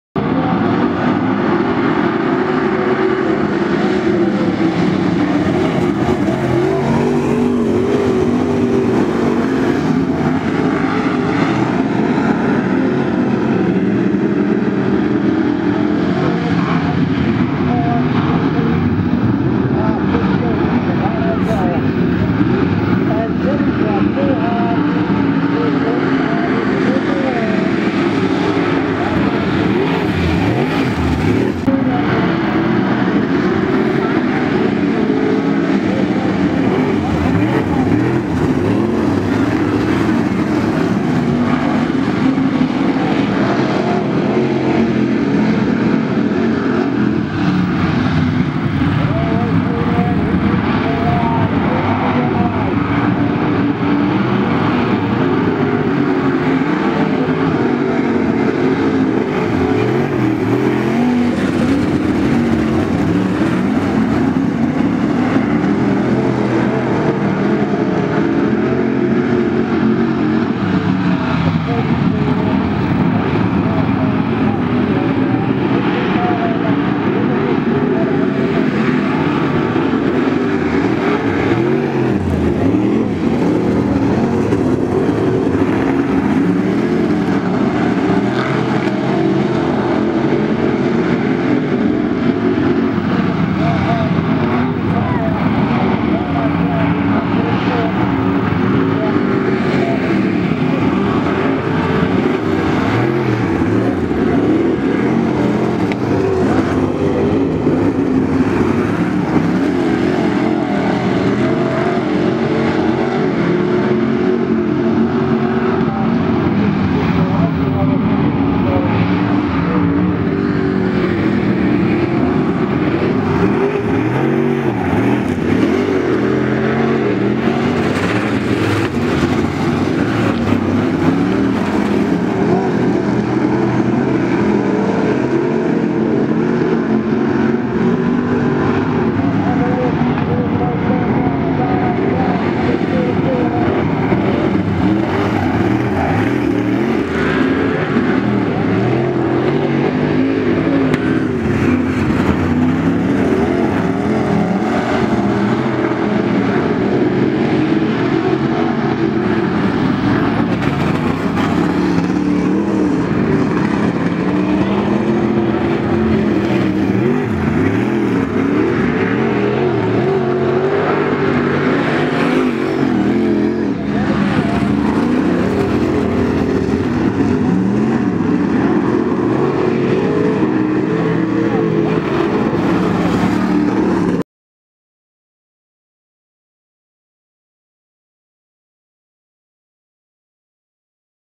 Ellenbrook Speedway Late Models Heat 4
May 26th 2013. Heat 4 - 1st Rob Galloway, 2nd Nevano Cowan and 3rd Mick Andrews.